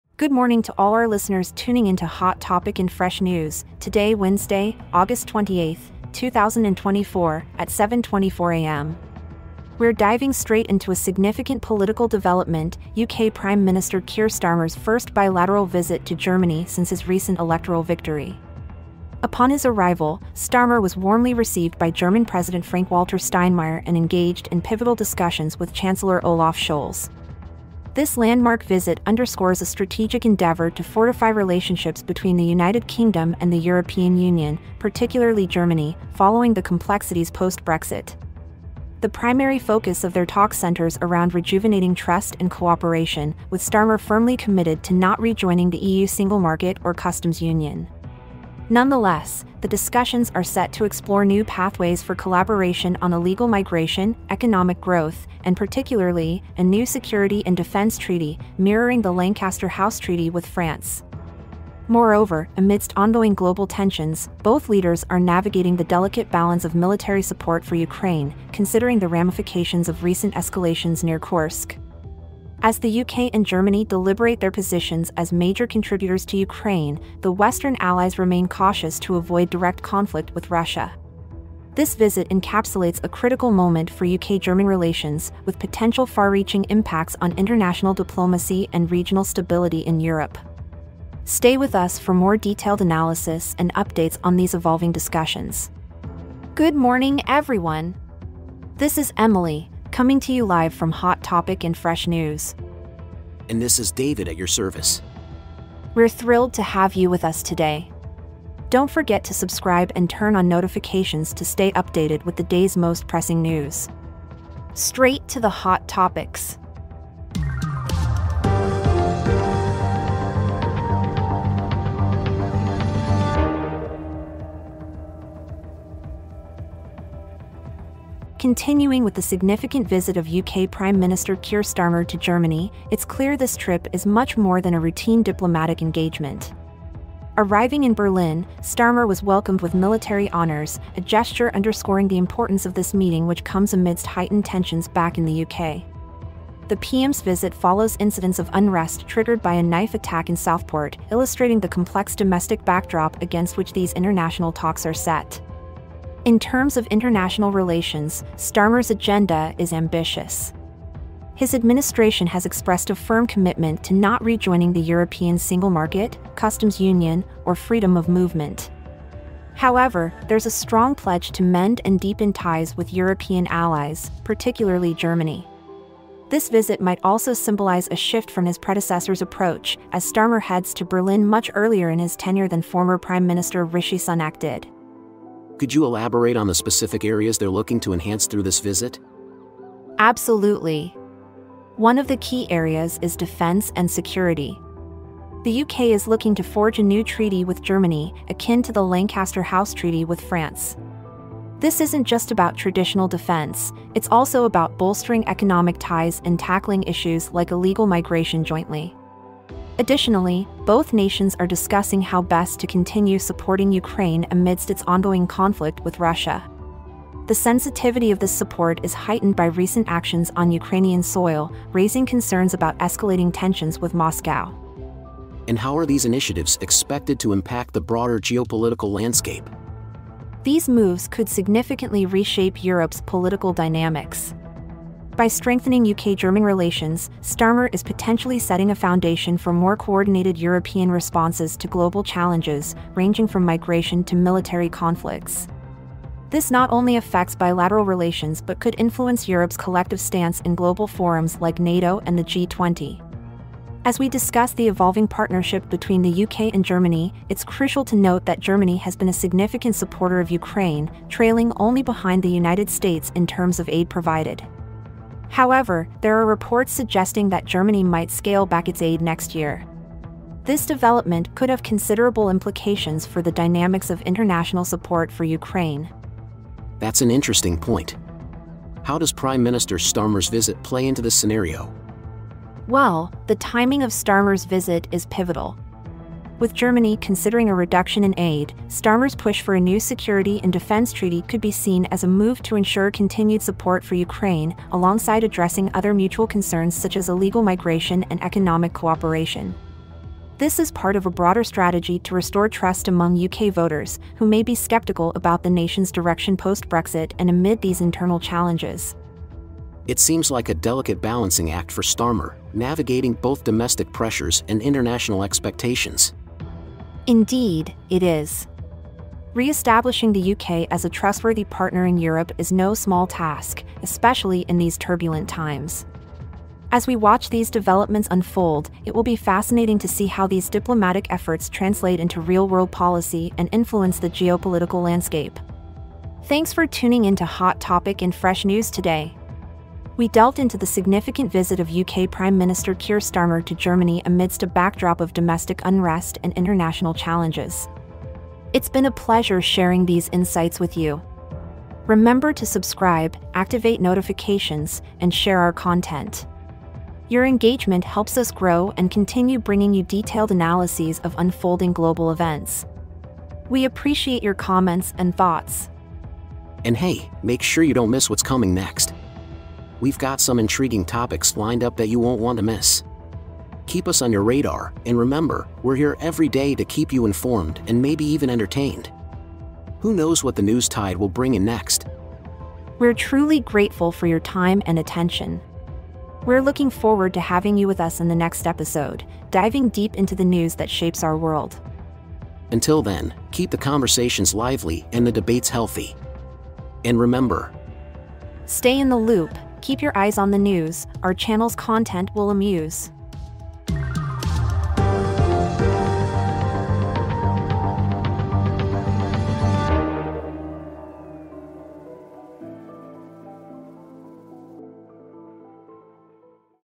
UK PM KEIR STARMER IN GERMANY: CRAFTING NEW DEFENSE TREATY!
🌍 UK Prime Minister Keir Starmer's Historic Visit to Germany! 🇬🇧🤝🇩🇪

In an unprecedented move, UK Prime Minister Keir Starmer embarks on a pivotal visit to Germany, aiming to fortify ties with one of Europe's leading powers. This video provides an exclusive inside look at the high-stakes discussions that could reshape the future of UK-German relations. 🌟 Discover how the new Labour government plans to draft a new security and defense treaty with Germany, marking a significant shift in international diplomacy.

Join us as we dive deep into the intricacies of this diplomatic venture. From intense negotiation sessions to strategic alliances, we cover all the bases, providing you with firsthand insights into the potential impacts of this treaty on global security dynamics. 📈 Plus, don't miss out on expert analyses and interviews with political pundits who break down what this means for the future of Europe and beyond.

Did you know? The UK and Germany have a longstanding history of cooperation but have faced strains in recent years due to geopolitical shifts and policy changes. This visit could potentially herald a new era of strengthened ties between the two nations. 📚

Your engagement helps us bring more such detailed analyses to you. 📹💬

Inspired by an article from: Deutsche Welle
Original title: UK Prime Minister Keir Starmer visits Germany